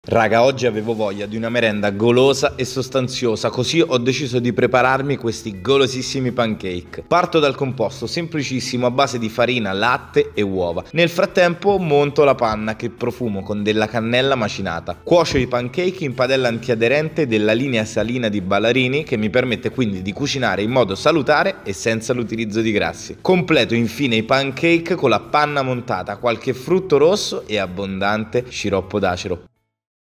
Pancake con panna alla cannella
Con l'arrivo dell'inverno c'è bisogno di merende più sostanziose e golose, che dite? Niente panico: arriva il nostro Chef Antonio in aiuto, con una ricetta semplice da preparare ma dal risultato sorpendente, oltre che salutare! 😎 Questi pancake farciti con panna montata soffice al profumo di cannella, sciroppo d'acero e frutti rossi, infatti non hanno bisogno di grassi per la cottura, perché - come ci mostra il nostro - sfruttiamo appieno l'elevata antiaderenza della nuova linea di padelle Salina Ballarini. Una vera bomba 😍
Ingredienti:

Per l’impasto:
Uova, 2;
Lievito in polvere per dolci,  6 g;
Zucchero semolato, 10 g;
Sale, un pizzico;
Olio di semi, un cucchiaio.

Per decorare:
Panna montata zuccherata, q.b.;
Cannella in polvere, 5 g;
Frutti di bosco vari, q.b.;
Sciroppo d'acero, q.b.